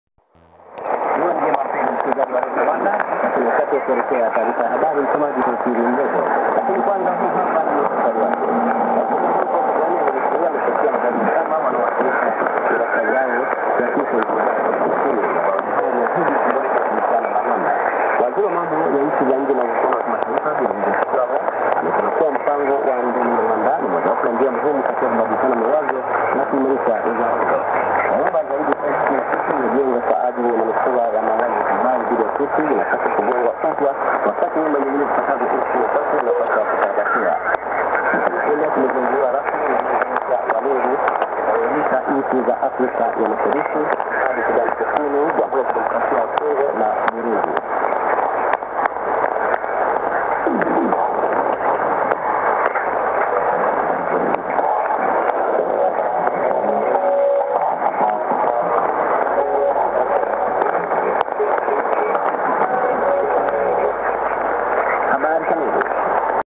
6055kHz Radio Rwanda Swahili programme ( 15:20UTC, Nov 28, 2013 )
The language is Swahili for sure,
Aerial: the build-in whip
QTH: Yokohama, Japan